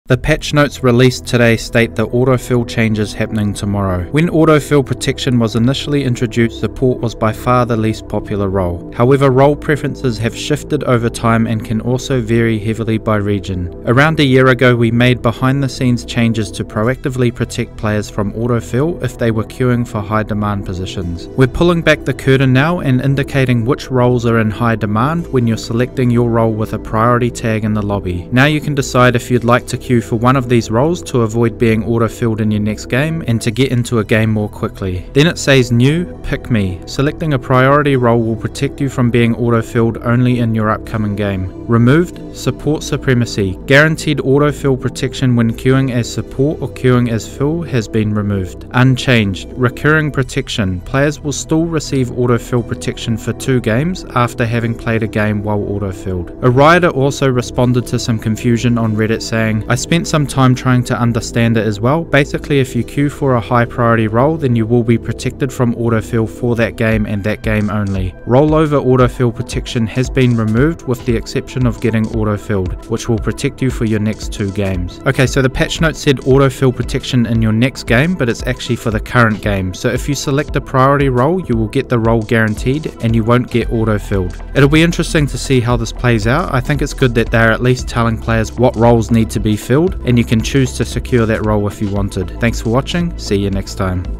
new ranked changes tomorrow
"When autofill protection was initially introduced, support was by far the least popular role. However, role preferences have shifted over time and can also vary heavily by region. Around a year ago we made behind-the-scenes changes to proactively protect players from autofill if they were queuing for high demand positions.

We’re pulling back the curtain now and indicating which roles are in high demand when you’re selecting your role with a “priority” tag in the lobby. Now you can decide if you’d like to queue for one of these roles to avoid being autofilled in your next game and to get into a game more quickly.

new PICK ME Selecting a priority role will protect you from being autofilled onlyin your upcoming game
REMOVED SUPPORT SUPREMACY Guaranteed autofill protection when queuing as support or queuing as fill has been removed
UNCHANGED RECURRING PROTECTION Players will still receive autofill protection for 2 games after having played a game while autofilled"